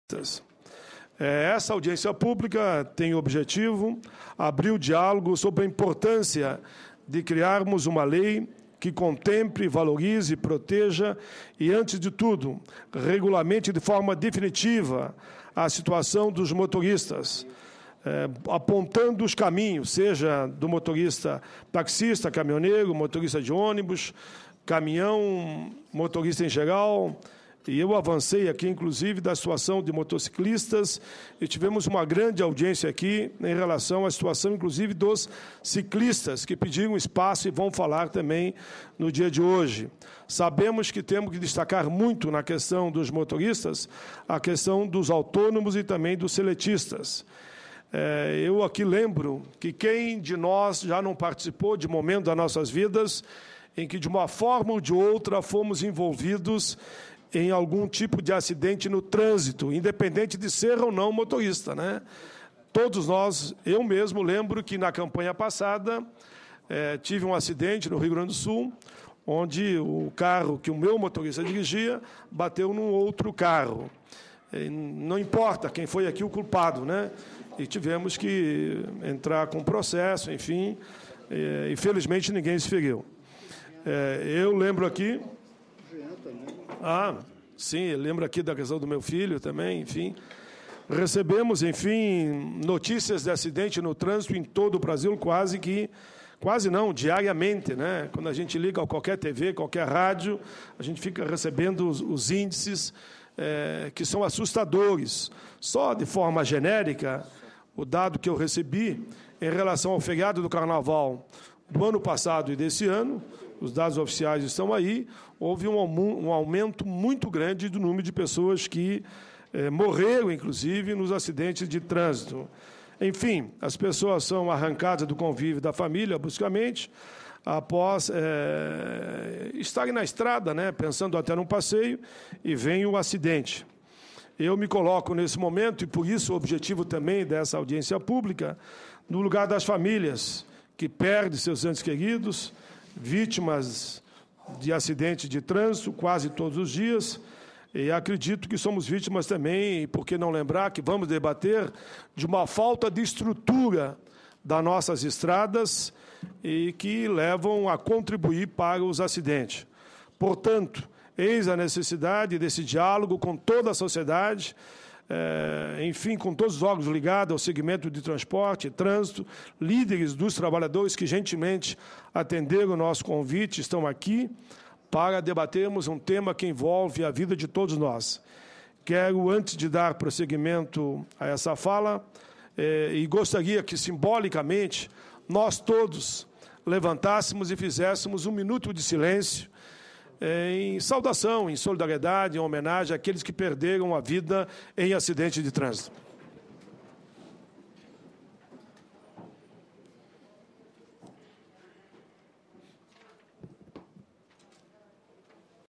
Comissão faz um minuto de silêncio em homenagem àqueles que perderam a vida em acidentes de trânsito
04/04/2011 - CDH - Comissão faz um minuto de silêncio em homenagem àqueles que perderam a vida em acidentes de trânsito